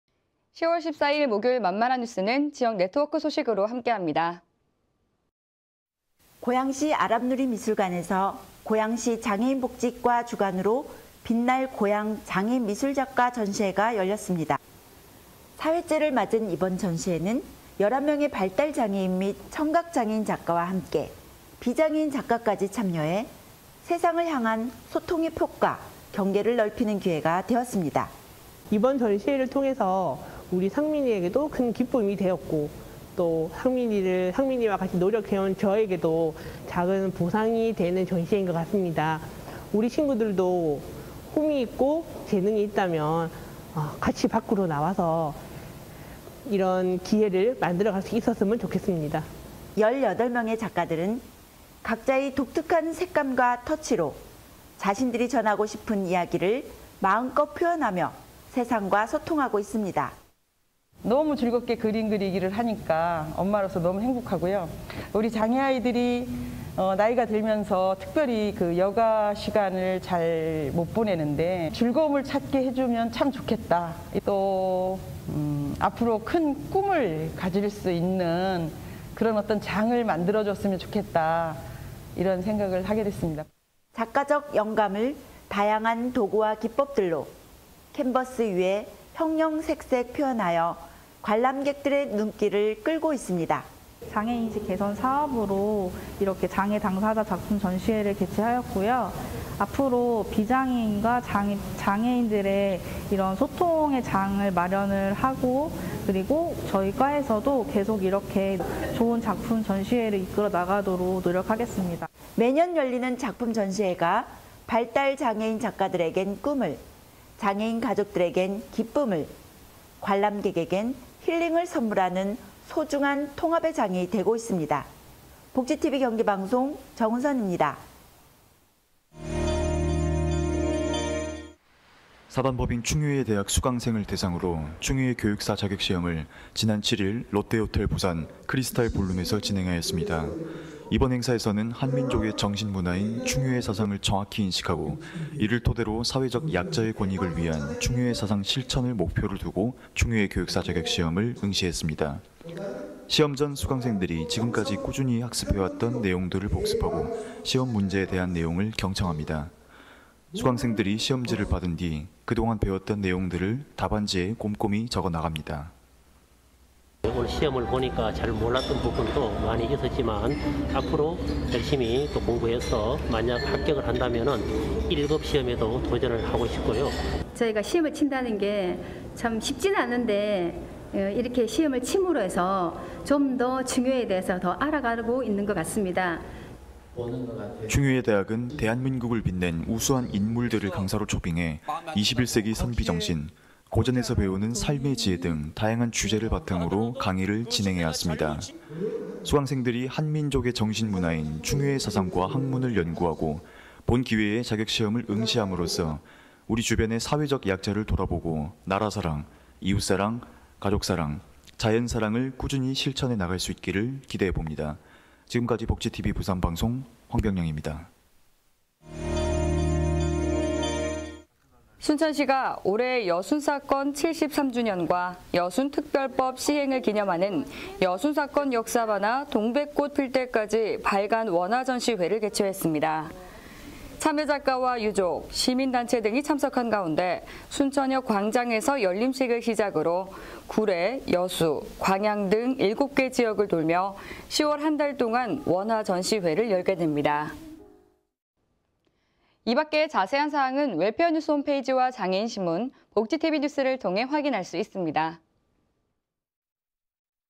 20211014 복지TV지역네트워크(만만한뉴스)_정체영상
20211014 복지TV지역네트워크(만만한뉴스)_정체영상

1_지역주요) 장애인 미술작가, 전시회로 세상과 소통_정운선 기자

2_지역주요) 충효예 대학 충효예교육사 자격시험 열려_황병양 기자

3_지역주요) 순천시, 여순사건 역사만화 발간기념 원화전시회 개최_전남